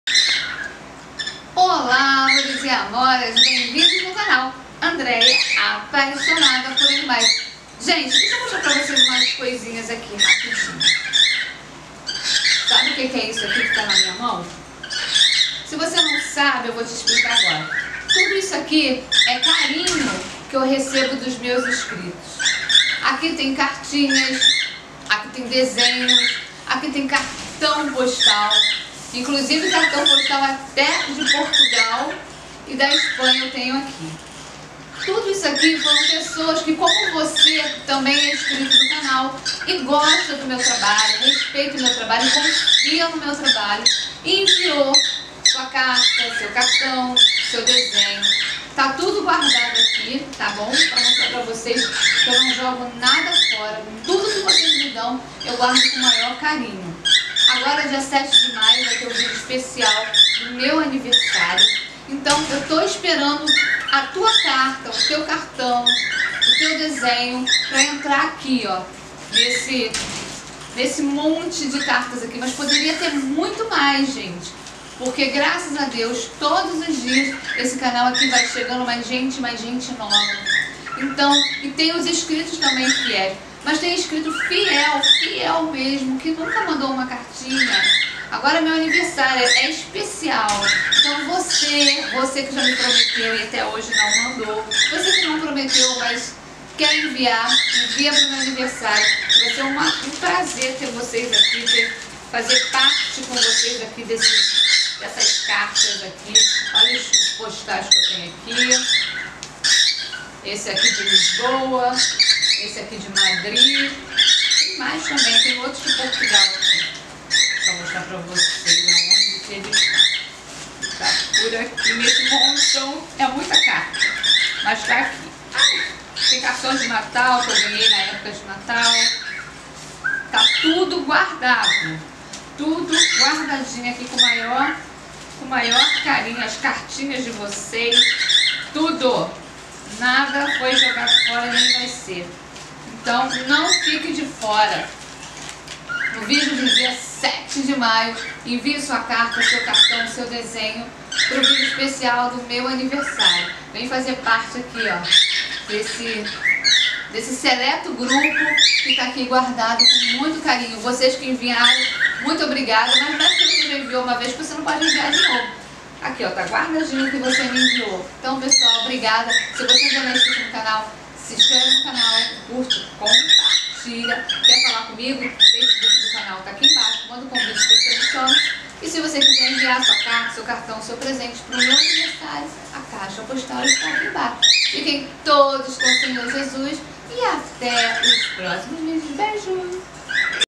ESTA CHEGANDO NÃO FIQUE DE FORA
CAIXA POSTAL 27038  CEP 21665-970   RJ/RJ   ANDREIA SILVA

OLÁ PESSOAS DO BEM, NO VÍDEO ESTA CHEGANDO NÃO FIQUE DE FORA FALO SOBRE O VÍDEO ESPECIAL QUE VAI TER NO DIA 7 DE MAIO.. ASSISTAM COMPLETO AO VÍDEO ESTA CHEGANDO NÃO FIQUE DE FORA E PARTICIPE VOCÊ TAMBÉM.  OBRIGADA